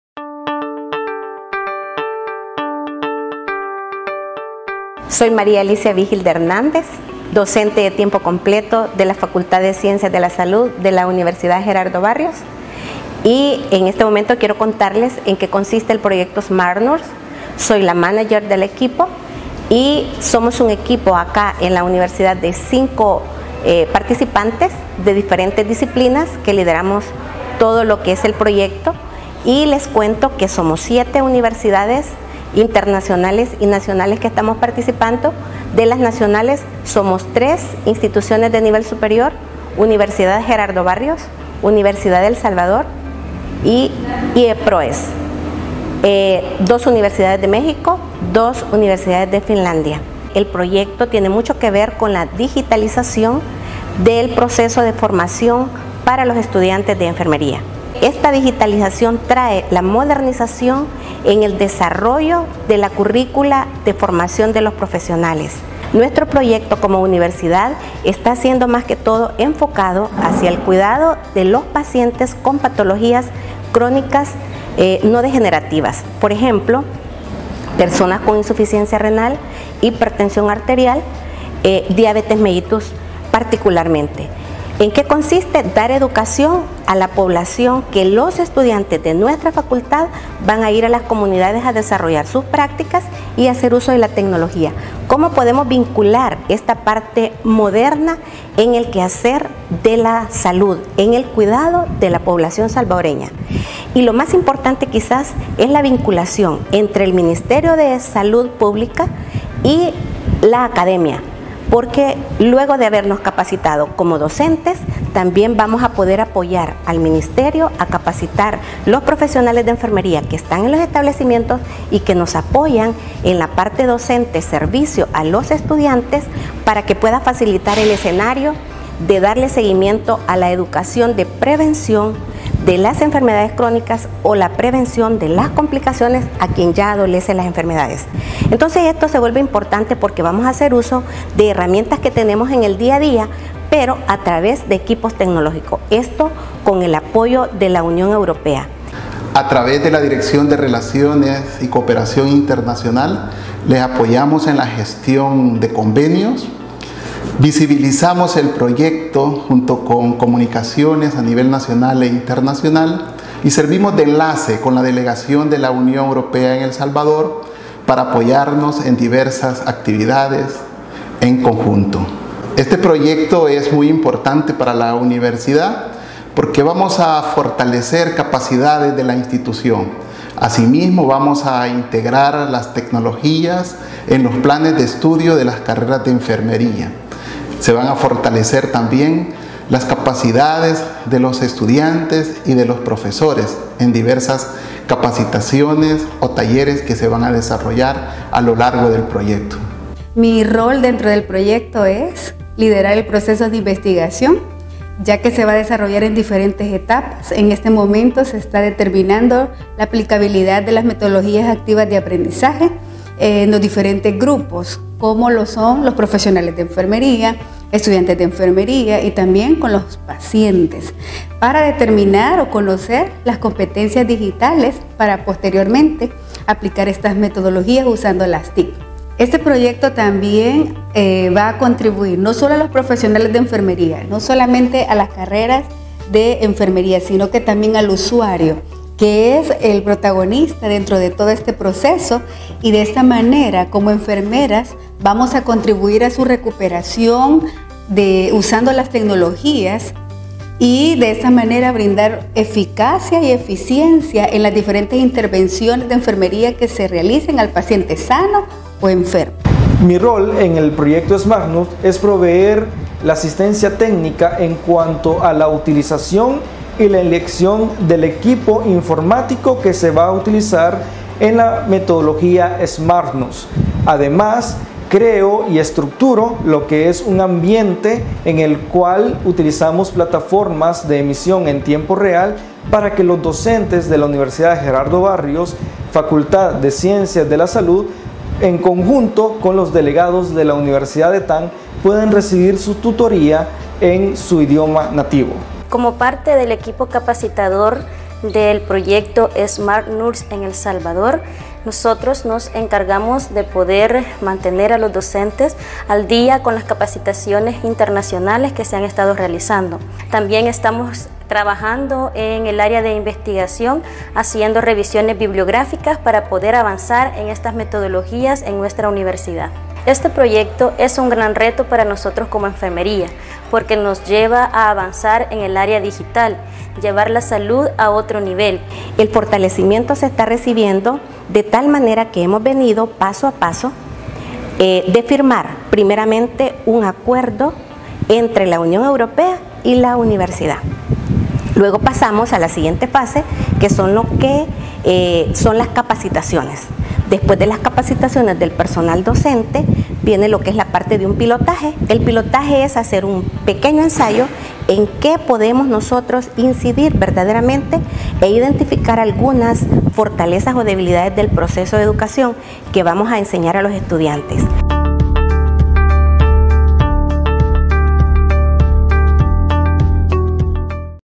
Proyecto Smart Nurse
El proyecto Smart Nurse es un el proyecto financiado por el Erasmus Plus de la UE. Este proyecto fortalece el currículo de las carreras de enfermería modernizando los planes de estudio e integrando las TICs al proceso de enseñanza.